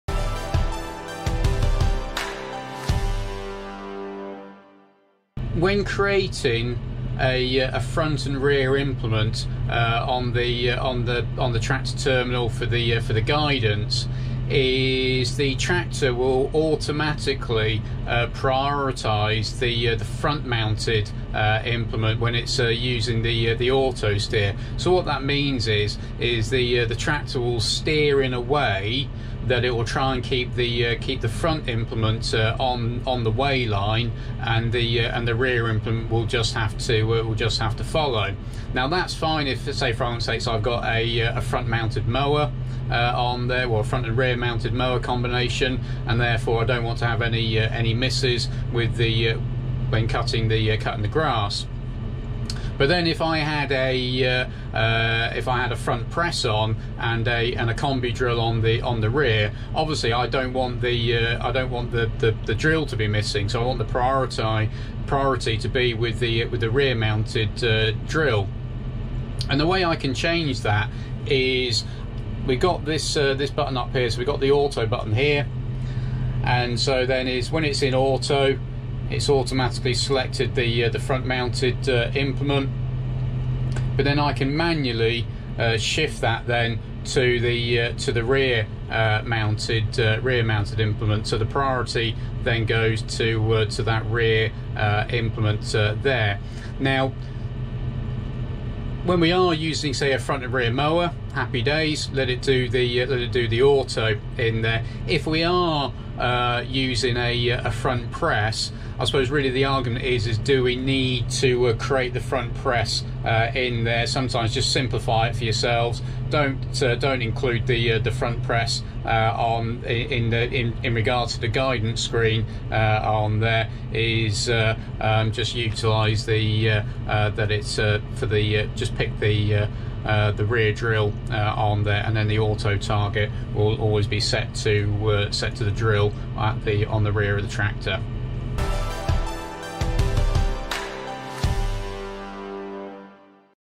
FendtOne Steering control target
Checking and altering the steering control target when using the guidance with a front mounted implement when using the FendtOne terminal